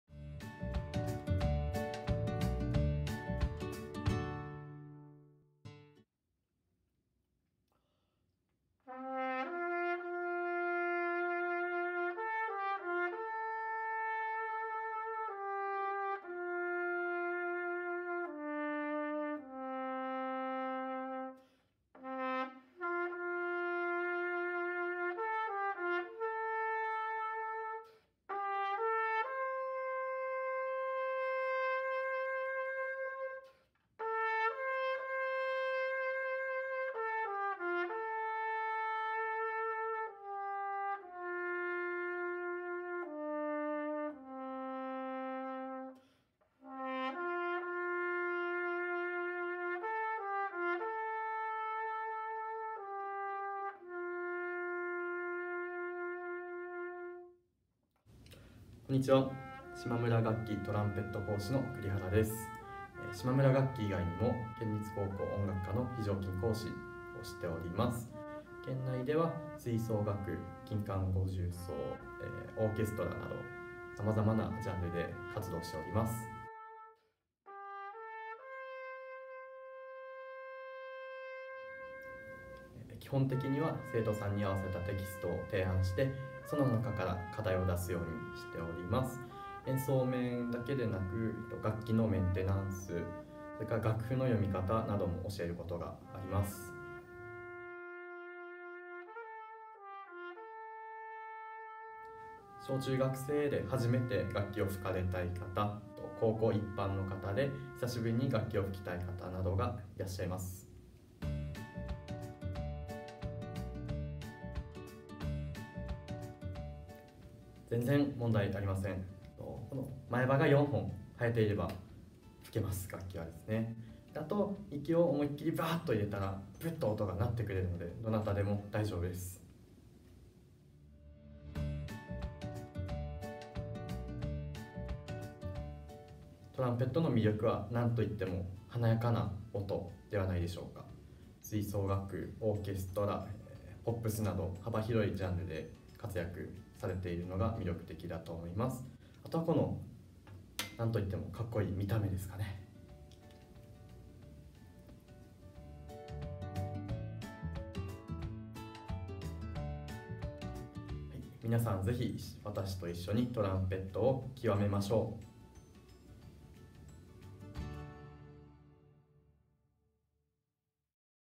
島村楽器 鹿児島アミュプラザ店 トランペット教室 講師紹介
島村楽器 音楽教室のトランペット科講師 栗原幸祐のご紹介です。
ただいまレッスン会員募集中！体験レッスンも随時受け付けております。

【島村楽器鹿児島アミュプラザ店・ミュージックサロン鹿児島】
■音楽教室総合ページ